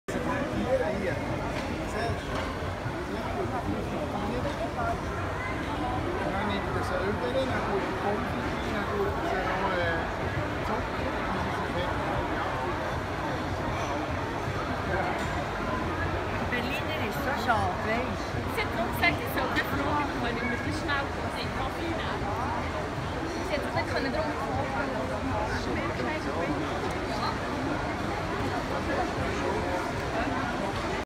スイス発 スイスでみんな大好き！ベルリーナー！ベルンのBEAで。【スイス情報.com】
ベルリーナーは、文字通り、ドイツの「ベルリンの人」という意味。ケネディ大統領が冷戦時代に「Ich bin ein Berliner」とベルリン市民の味方という意味で発言したことで有名ですが、動画の「Berliner」はイチゴジャム入りの揚げパンです！TT